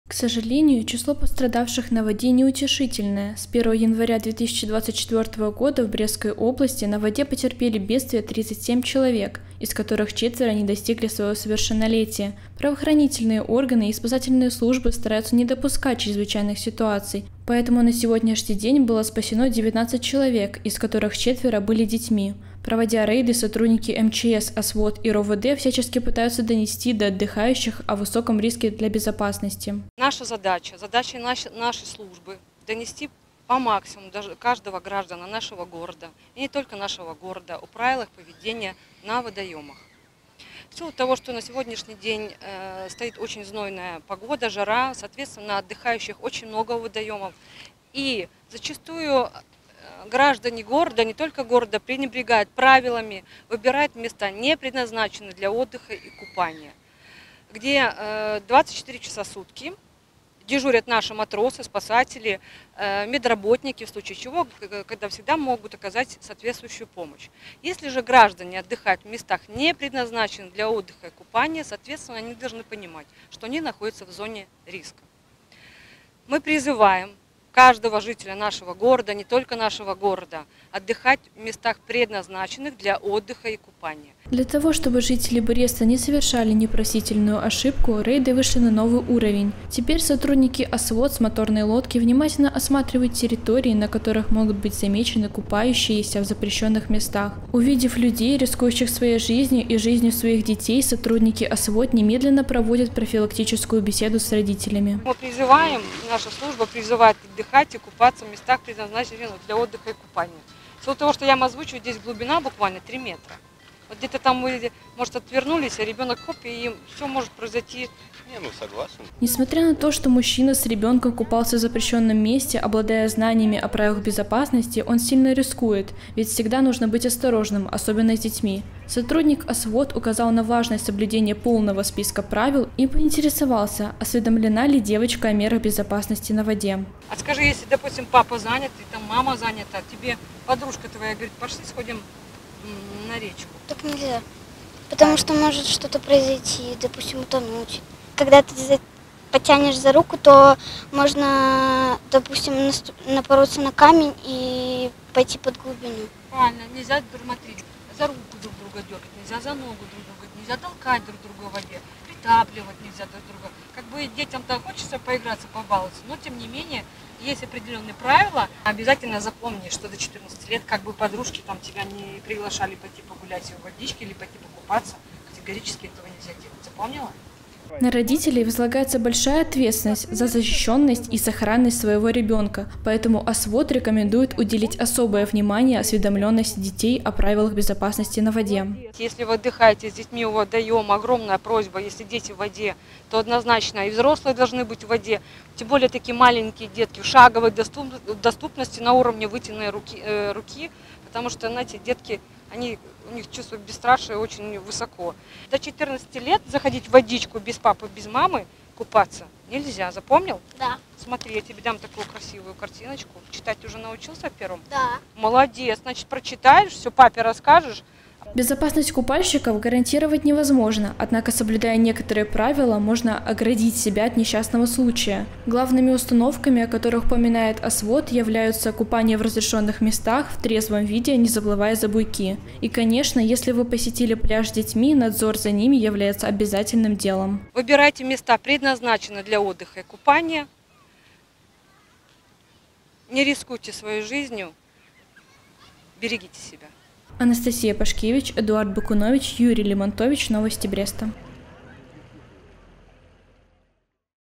2024-07-18 Рейд ОСВОД по несанкционированным местам отдыха
Знают ли Брестчане о правилах безопасности на воде – знают, вопрос в другом – исполняют ли их? -  трудно сказать. И вот в данном случае, чтобы обезопасить жизни отдыхающих, к своим прямым служебным обязанностям приступают сотрудник ОСВОДа, МЧС, РОВД. Именно они берут на себя ответственность за безопасный отдых на воде. Формы и методы - самые различные: от профилактических бесед, раздачи   листовок с правилами поведения на воде до административной ответственности. В этот раз рейд проходил с плавающего судна с проверкой акваторий несанкционированных пляжей. Анастасия Пашкевич расскажет, чем мотивировали люди, отдыхающие та, где не нужно отдыхать!

*Наши контакты:*
✅Телефон горячей линии:
54-63-67